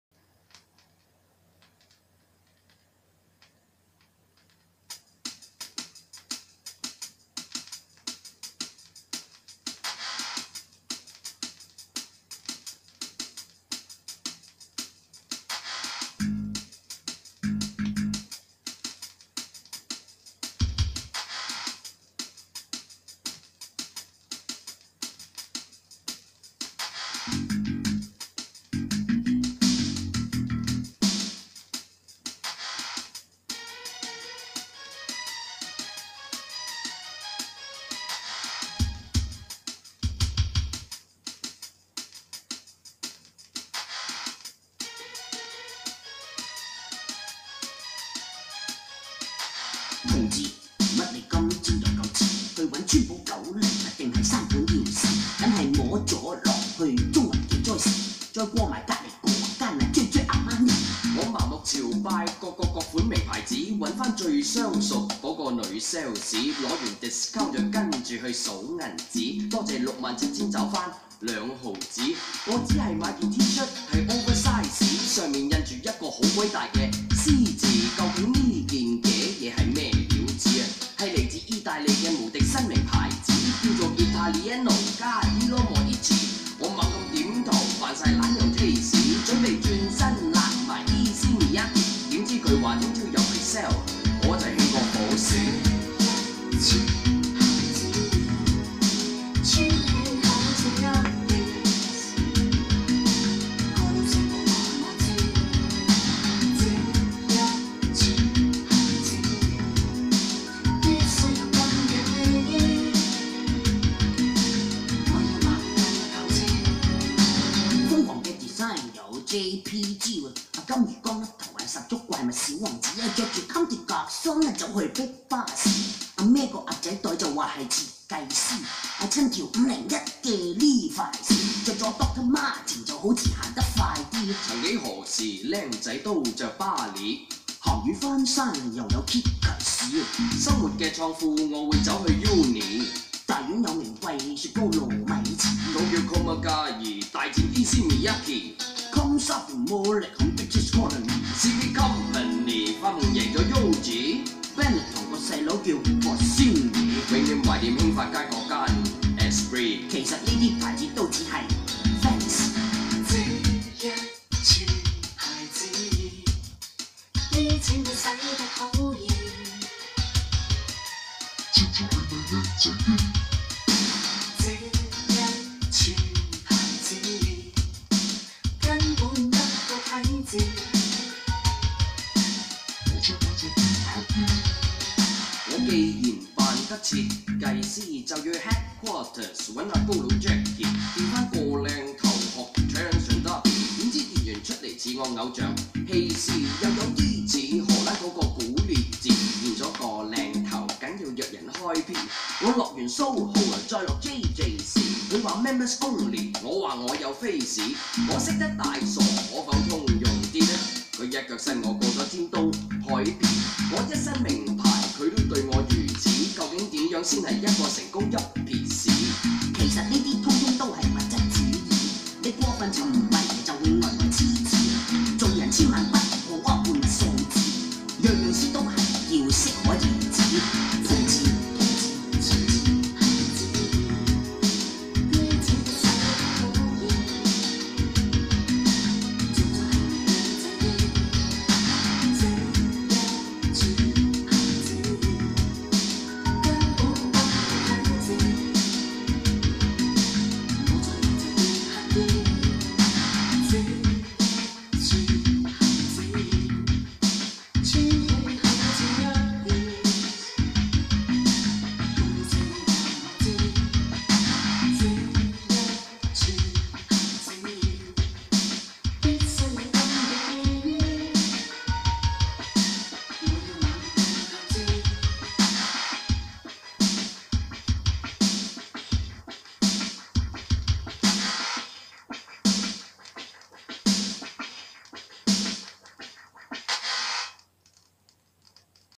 Demo for 軟硬 -- 黑膠 -- HiFi
NOT for download, thx !
當年音響展，麥記的打碟員話呢隻係試音碟，
其時佢係播 CDR．．麥記聞名列式單元音箱，
4 只低音無數中高音一聲道，印象中震的我身軀移動，
今時今日再聽黑膠版，真的可以試音的！